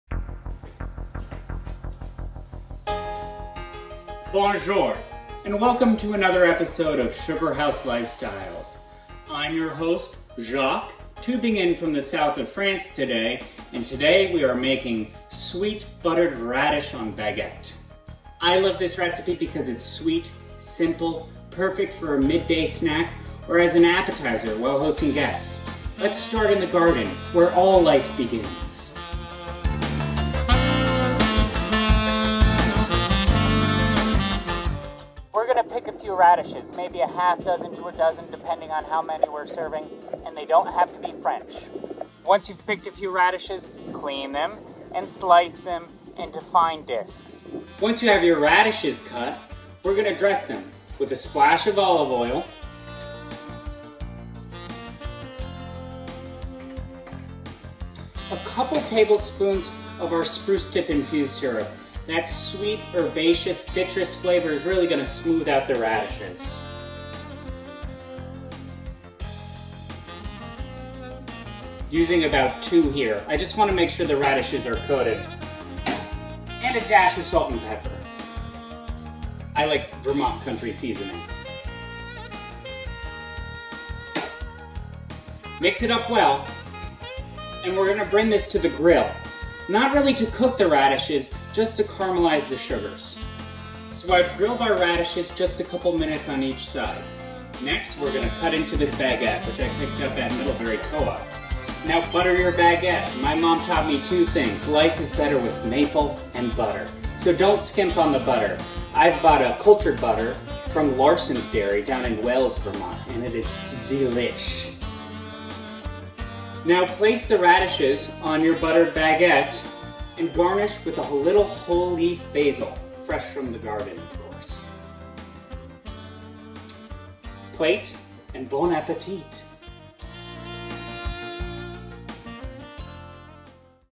Sugarhouse Lifestyles Ep 10
Explore other ways to use maple syrup! Check out this fun and easy summer recipe.